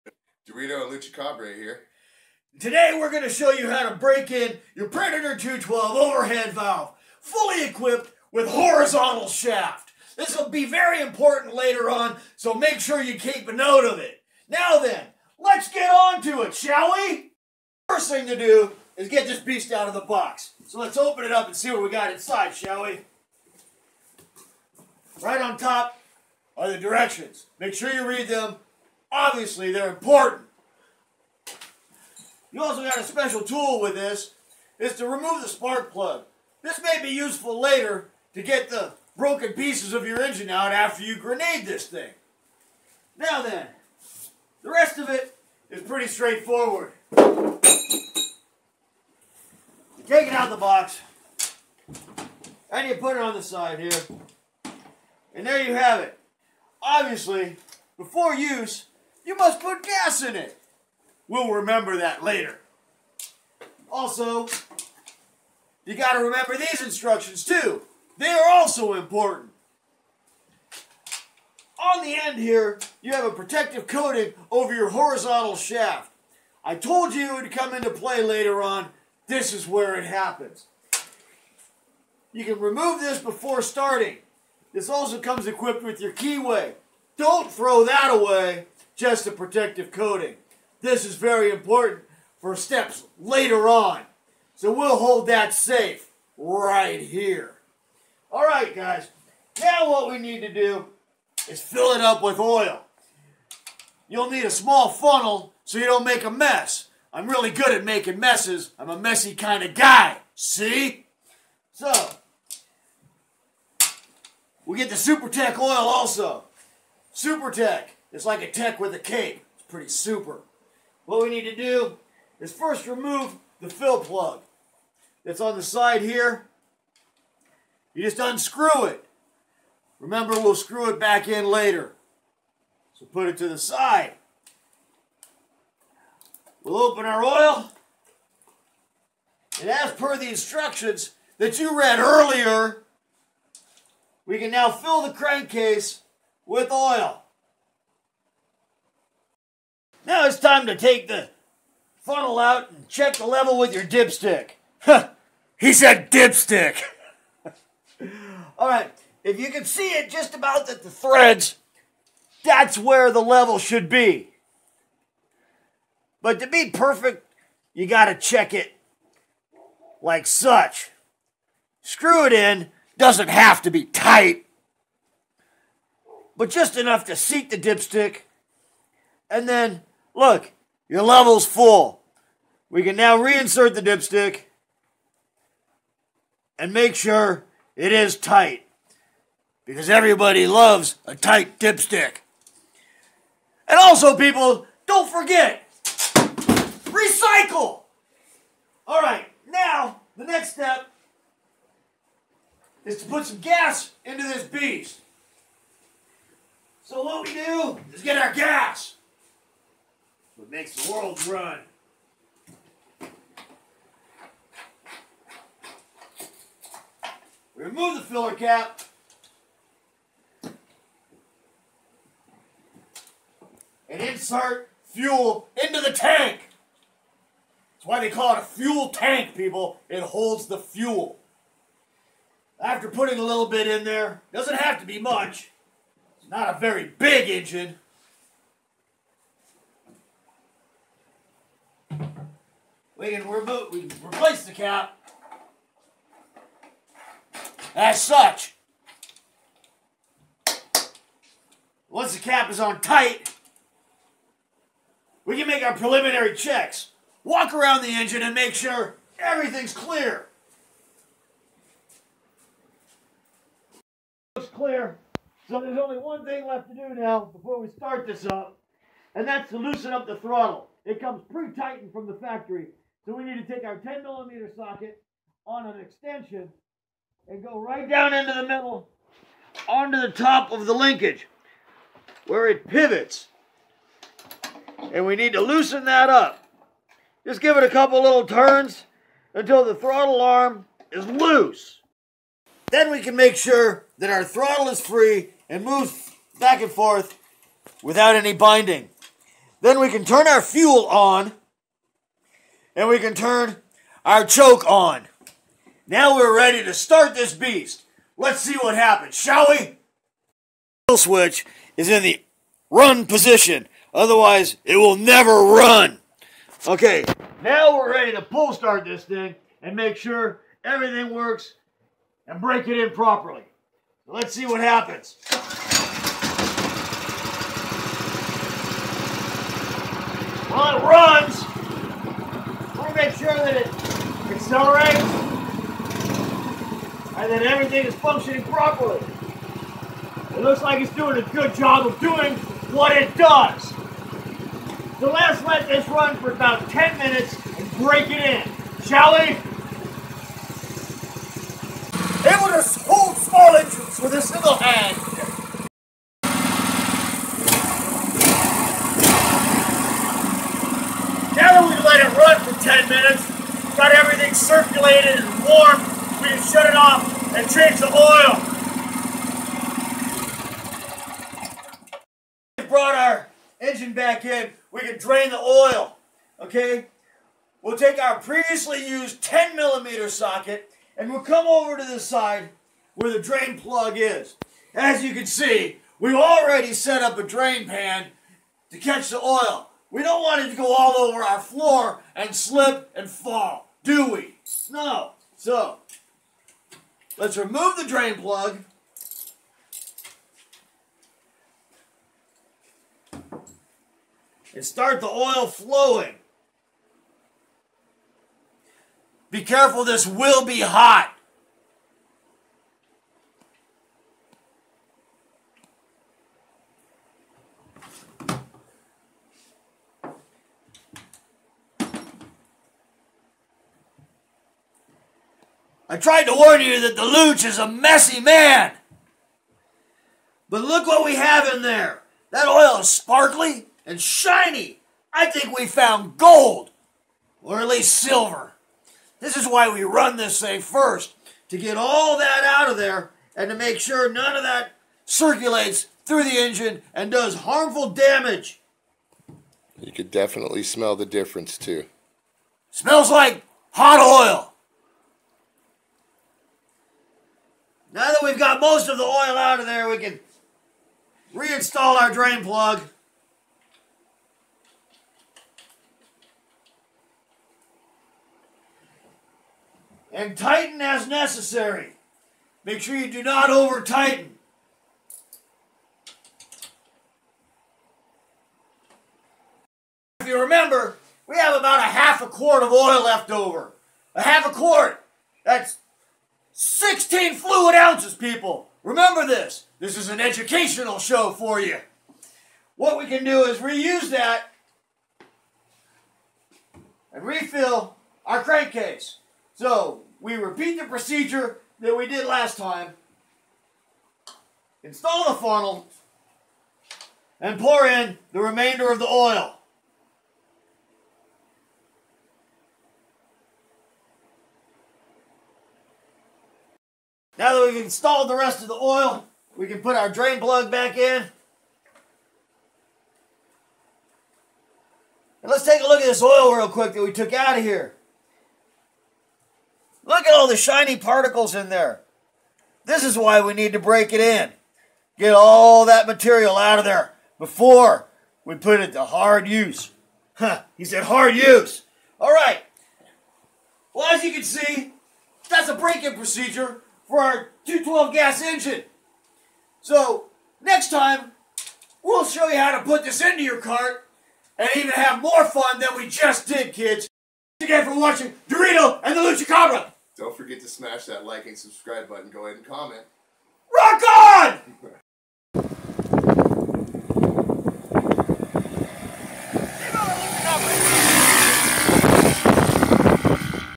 PREDATOR 212 BREAK IN
How to do your the FIRST Break in and oil change on a Harbor Frieght PREDATOR 212 cc engine